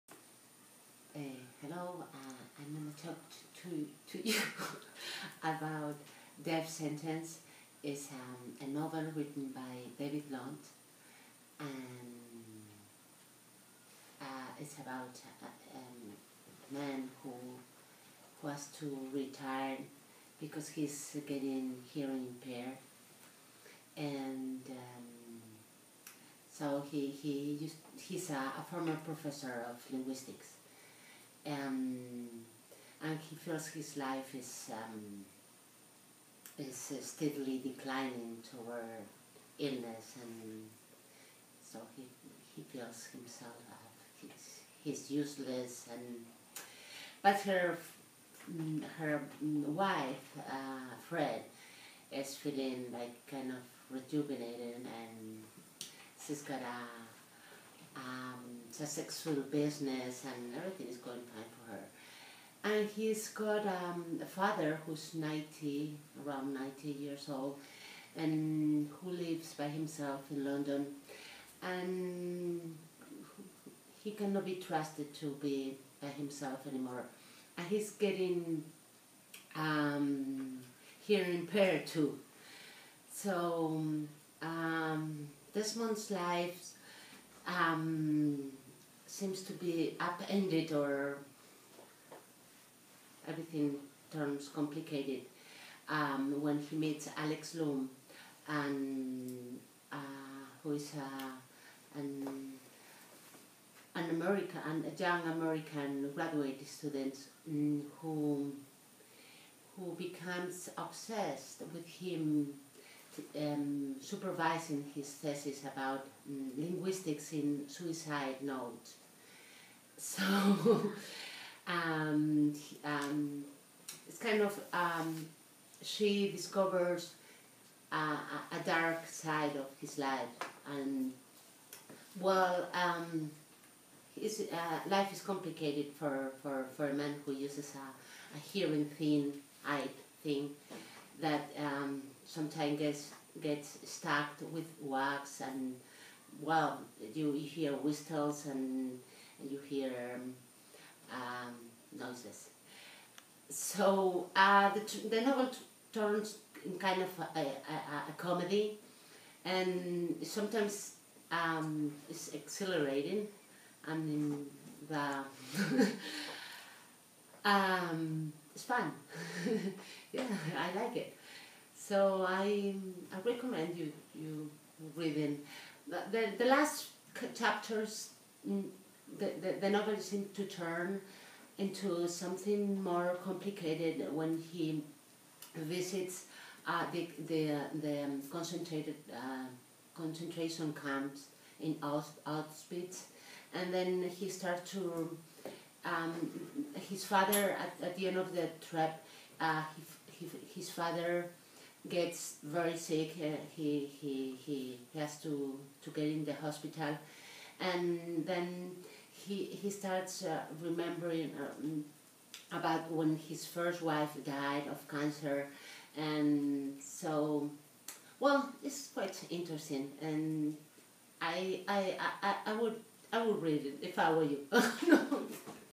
Deaf sentence, starring Mercedes
Mercedes, a B2 ESL student at EOI Luarca, reviews this book by David Lodge.